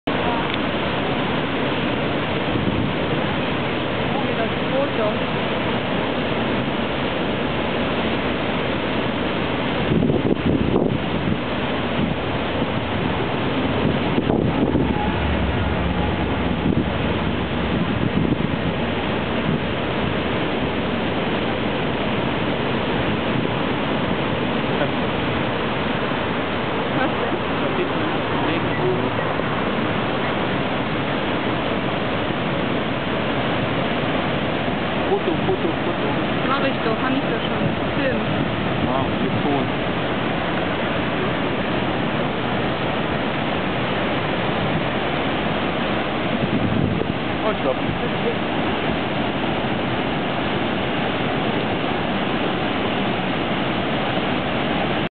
Niagarafalls,September 2007, Part 3
I was so thrilled by these waterfalls I couldn't stop filming them.